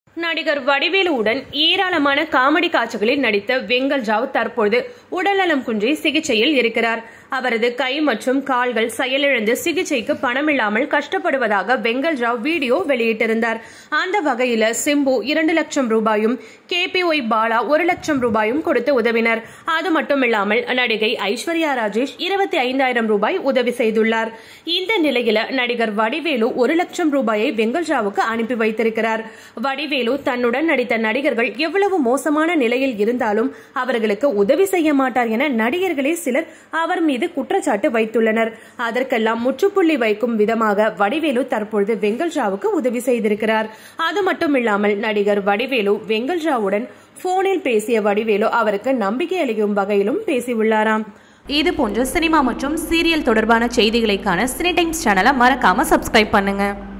நடிகர் வெங்கல் ராவுக்கு உதவிய வடிவேலு!
நடிகர் வடிவேலு உடன் ஏராளமான காமெடி காட்சிகளில் நடித்த வெங்கல் ராவ் தற்போது உடல்நலம் குன்றி சிகிச்சையில் இருக்கிறார். இந்நிலையில் நடிகர் வடிவேலு வெங்கல் ராவுக்கு  பணம் அனுப்பி வைத்திருக்கிறார்.